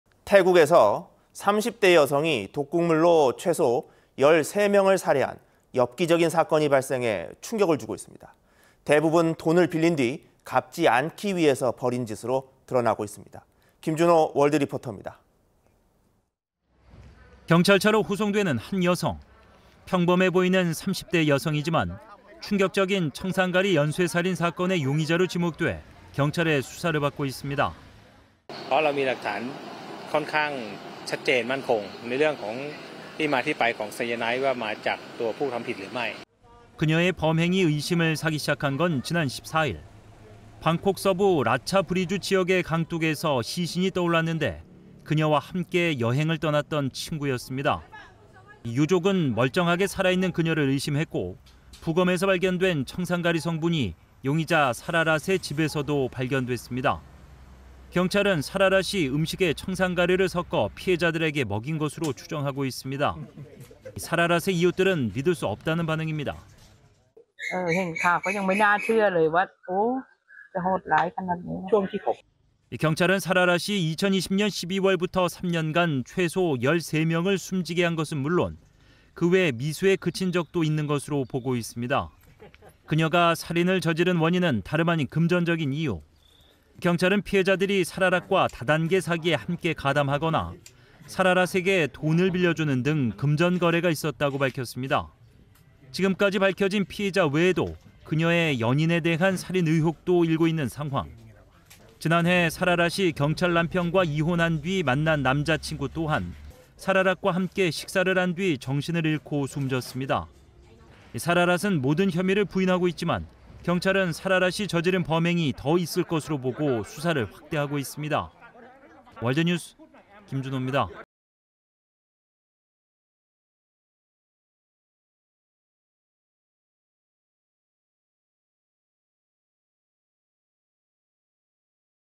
태국 30대 여성.. 청산가리로 13명 연쇄살인 / OBS 오늘의 월드뉴스
【앵커】
태국에서 30대 여성이 독극물로 최소 13명을 살해한 엽기적인 사건이 발생해 충격을 주고 있습니다.
대부분 돈을 빌린 뒤 갚지 않기 위해서 벌인 짓으로 드러나고 있습니다.
김준호 월드리포터입니다.

【기자】

경찰차로 호송되는 한 여성.

평범해 보이는 30대 여성이지만 충격적인 청산가리 연쇄살인 사건의 용의자로 지목돼 경찰의 수사를 받고 있습니다.

[아네크 타오스수파르프 / 경찰 담당 부서장 : 청산가리와 용의자와의 연관성을 입증할 상당히 명확한 증거를 확보했습니다.]

그녀의 범행이 의심을 사기 시작한 건, 지난 14일.

방콕 서부 랏차부리주 지역의 강둑에서 시신이 떠올랐는데, 그녀와 함께 여행을 떠났던 친구였습니다.

유족은 멀쩡하게 살아있는 그녀를 의심했고, 부검에서 발견된 청산가리 성분이 용의자 사라랏의 집에서도 발견됐습니다.

경찰은 사라랏이 음식에 청산가리를 섞어 피해자들에게 먹인 것으로 추정하고 있습니다.

사라랏의 이웃들은 믿을 수 없다는 반응입니다.

[폰팁 스리왈렛 / 용의자 이웃 : 뉴스도, 사라랏이 그렇게 끔찍한 일을 저지른 것도 믿어지지 않았어요.]

경찰은 사라랏이 2020년 12월부터 3년간 최소 13명을 숨지게 한 것은 물론 그외 미수에 그친 적도 있는 것으로 보고 있습니다.

그녀가 살인을 저지른 원인은 다름 아닌 금전적인 이유.

경찰은 피해자들이 사라랏과 다단계 사기에 함께 가담하거나 사라랏에게 돈을 빌려주는 등 금전 거래가 있었다고 밝혔습니다.

지금까지 밝혀진 피해자 외에도 그녀의 연인에 대한 살인 의혹도 일고 있는 상황.

지난해 사라랏이 경찰 남편과 이혼한 뒤 만난 남자친구 또한 사라랏과 함께 식사를 한 뒤 정신을 잃고 숨졌습니다.

사라랏은 모든 혐의를 부인하고 있지만, 경찰은 사라랏이 저지른 범행이 더 있을 것으로 보고 수사를 확대하고 있습니다.

월드뉴스 김준호입니다.